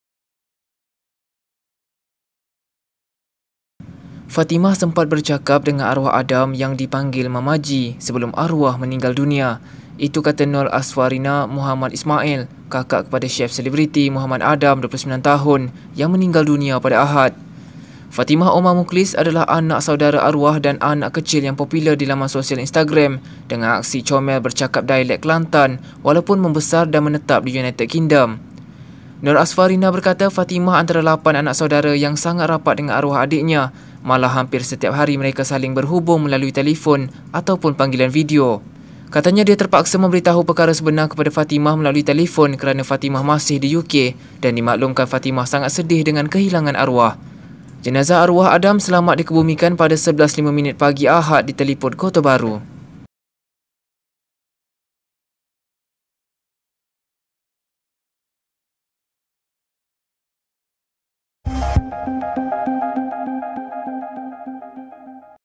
Fatimah London sempat bercakap dengan Mamaji
Fatimah sempat bercakap dengan arwah Adam yang dipanggil Mamaji sebelum arwah meninggal dunia.

Itu kata Nor Ashfarina Mohamed Ismail, kakak kepada chef selebriti, Mohamad Adam, 29, yang meninggal dunia pada Ahad.